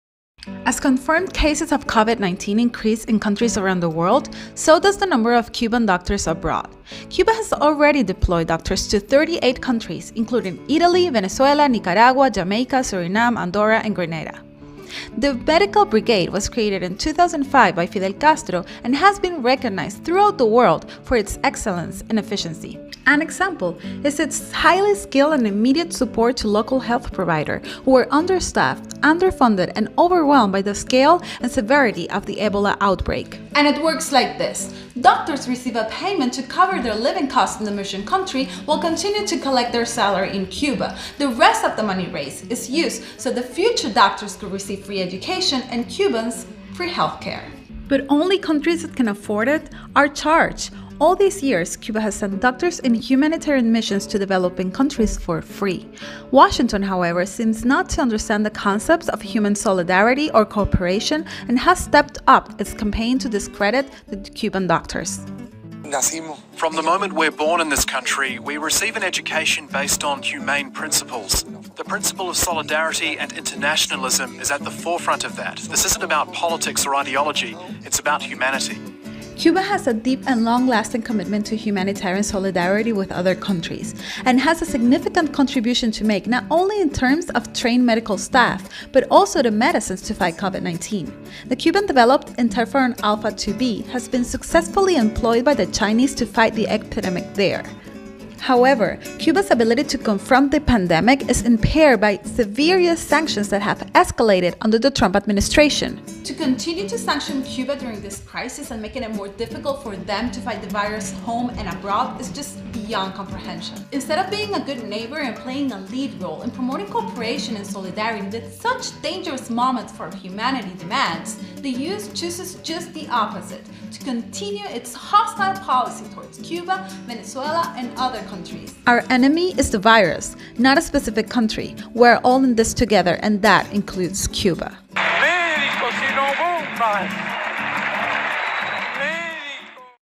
Cuban heroes
As the number of COVID-19 cases increase around the world, so does the number of Cuban doctors helping countries in need. Isn't time to end the criminal sanctions against Cuba? Bandera de Cuba